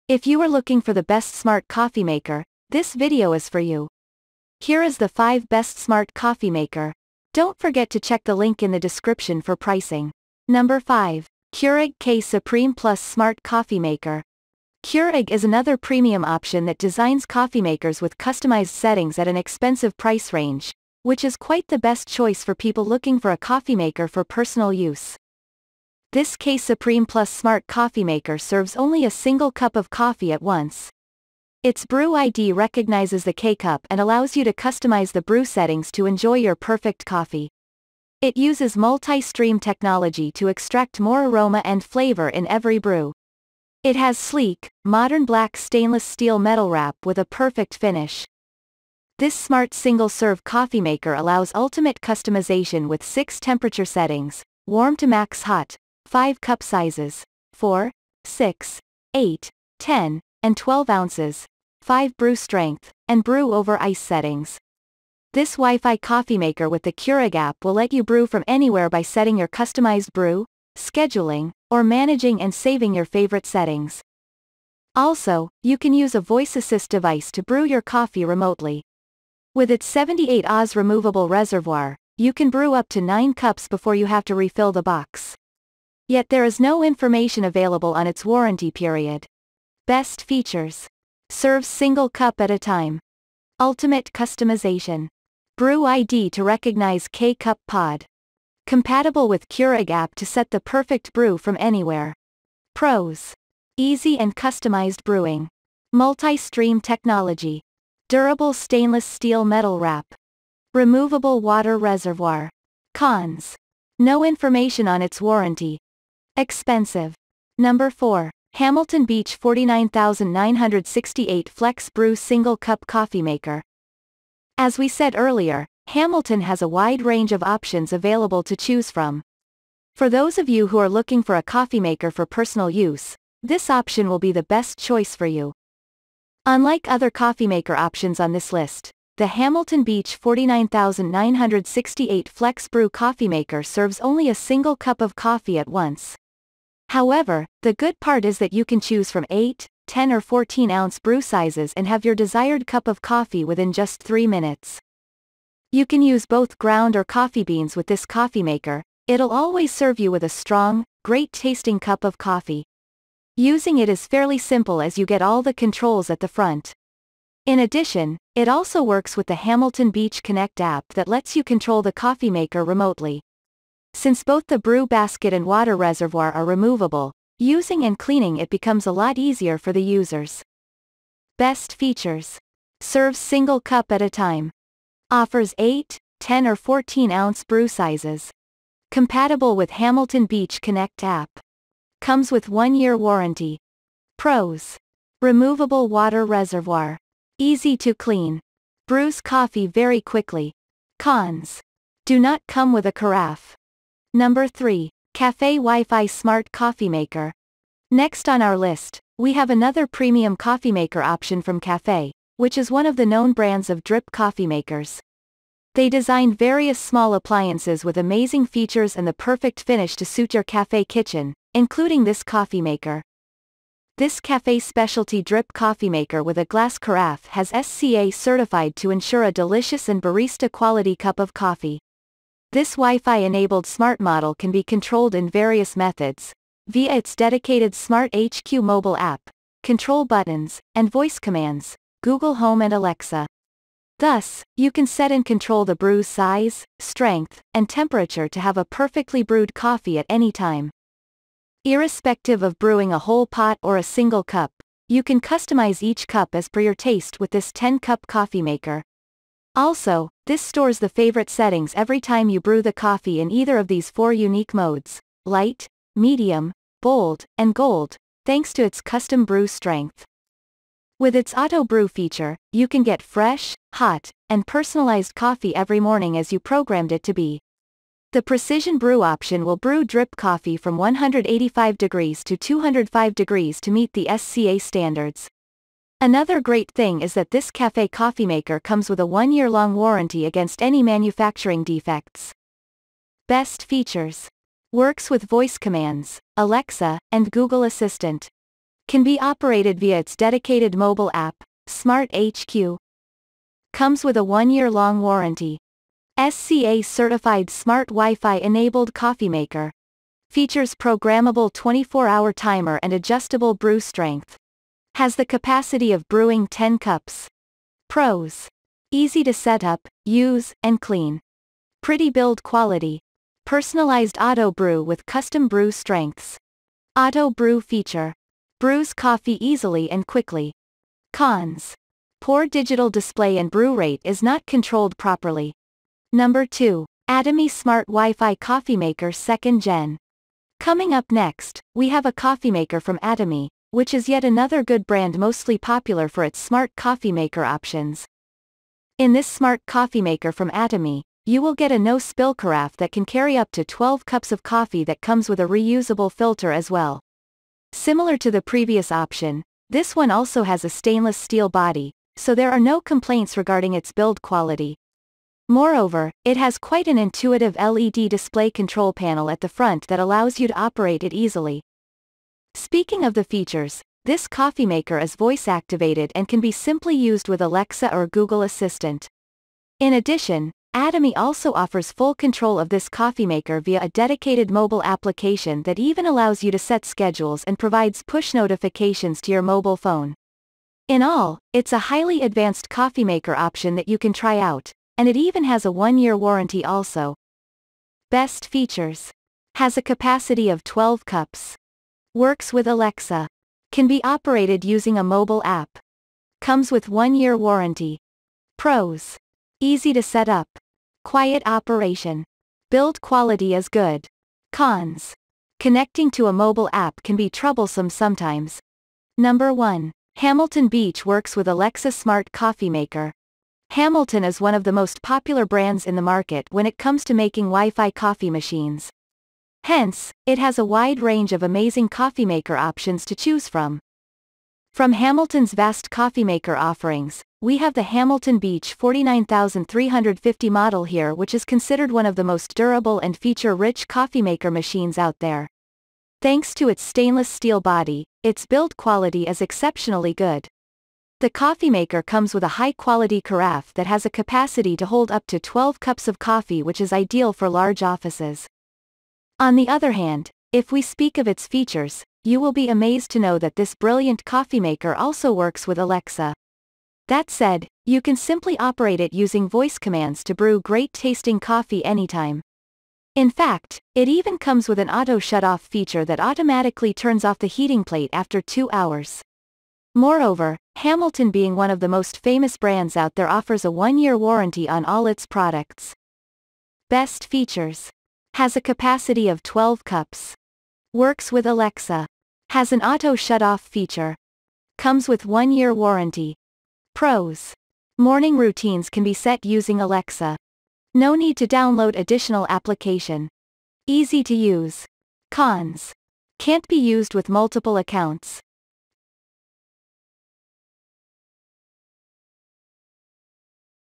Top 5 Best Smart Coffee Maker Reviews in 2023
5. Keurig K-Supreme Plus
4. Hamilton Beach 49968
3. Café Wi-Fi
2. Atomi Smart WiFi
1. Hamilton Beach Works